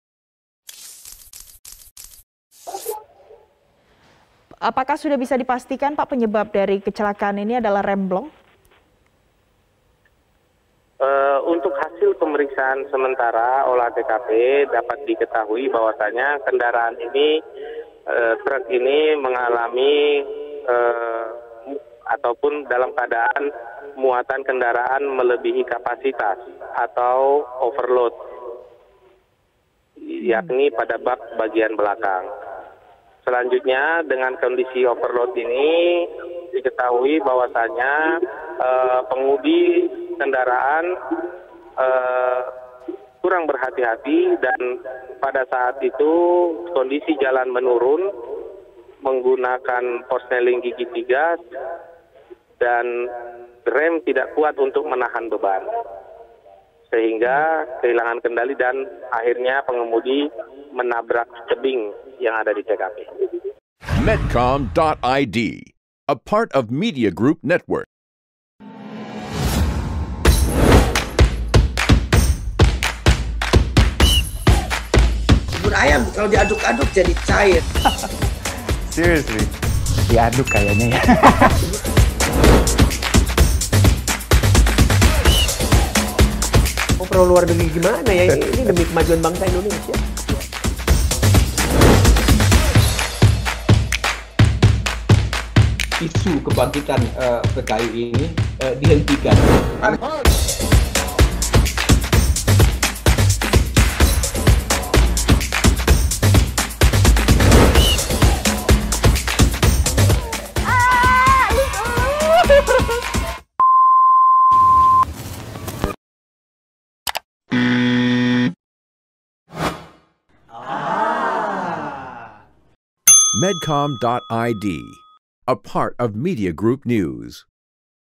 Penyebab Kecelakaan Maut di Pegunungan Arfak yang Menewaskan 16 Orang
Sebanyak 16 orang meninggal dan 13 orang dalam kondisi kritis dalam kecelakaan maut sebuah truk di Kilometer 10, Distrik Minyambou, Kabupaten Pegunungan Arfak, Papua Barat.

Kapolres Manokwari AKBP Parasian Herman Gultom mengatakan dari hasil olah TKP diketahui bahwa truk dalam keadaan kelebihan kapasitas pada bagian belakang. Selanjutnya, pengemudi juga dinilai kurang berhati-hati lantaran menggunakan gigi 3 pada jalan yang menurun. Sehingga, dengan kondisi yang kelebihan kapasitas saat mengerem truk tidak kuat menahan bebas dan pengemudi kehilangan kendali hingga akhirnya menabrak sebuah tebing.